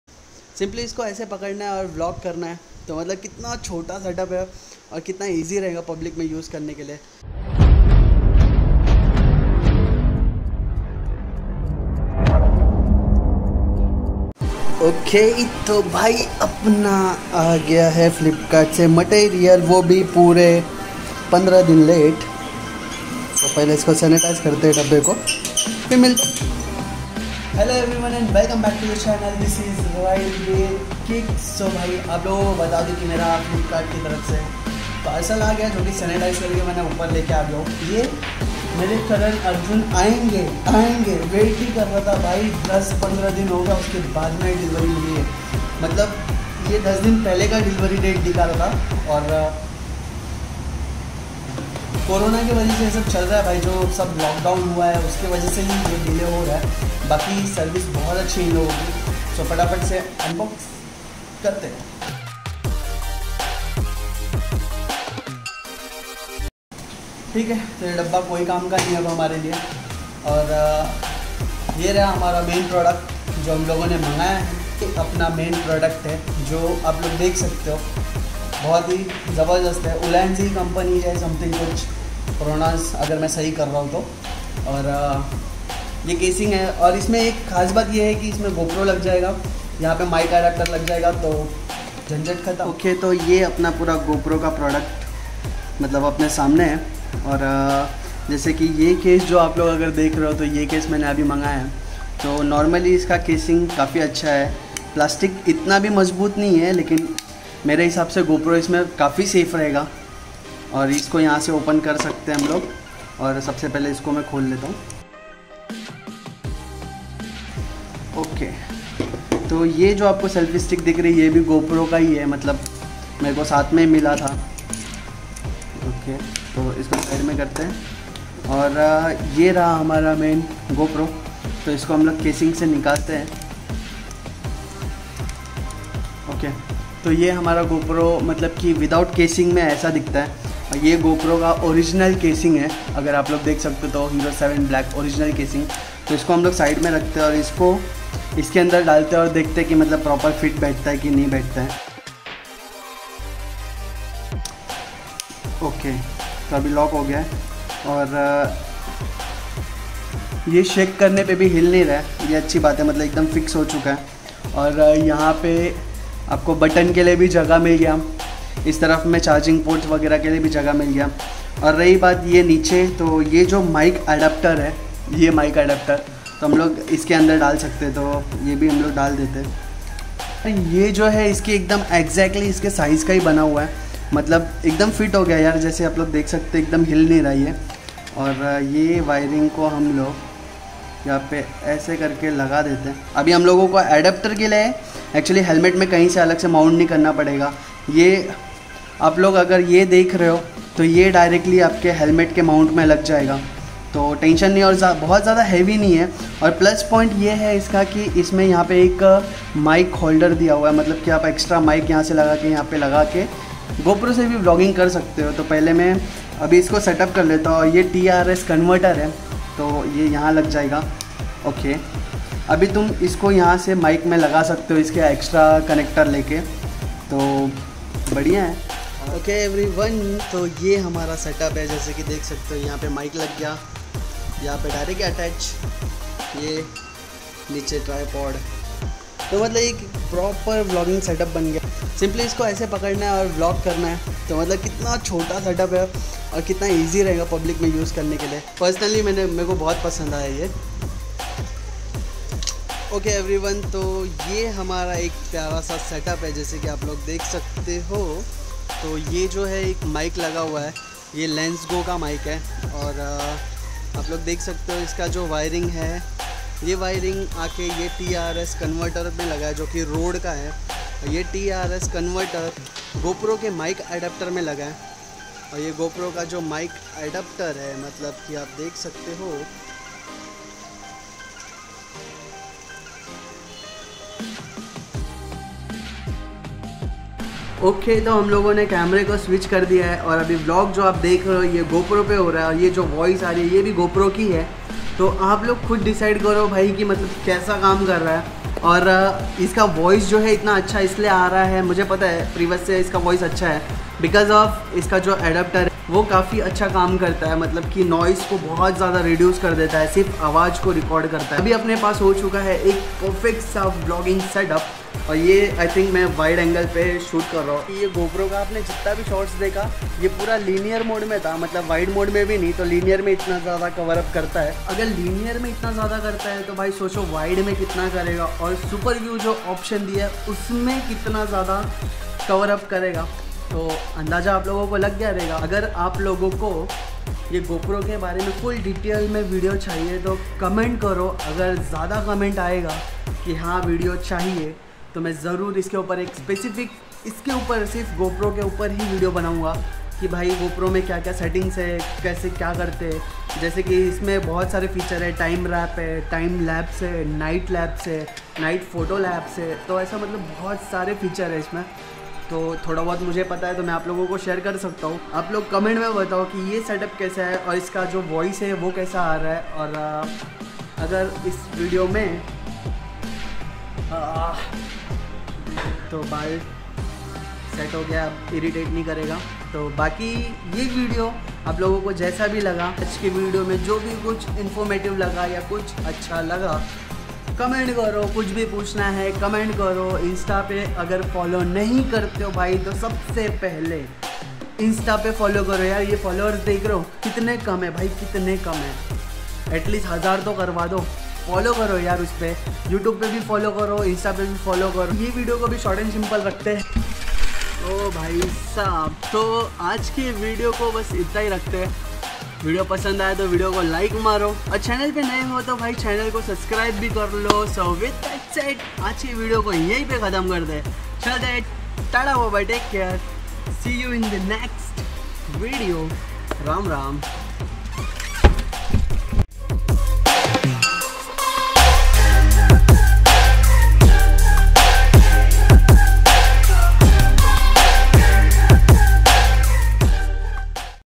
Ulanzi Case | For GoPro Hero 7 Black | Mic Adaptor Problem Solved | Ride With Kick
product - Take a look at this Action Pro Front & Back Case for GoPro 7 6 5 on Flipkart

HELLO GUYS